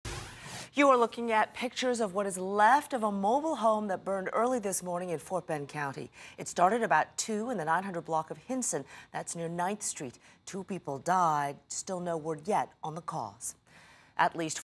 2 dead in mobile home fire in Richmond area
The Fort Bend County Sheriff's Office has confirmed that two people died overnight in a fire at a home located in the 900 block of Hinson in the Richmond area.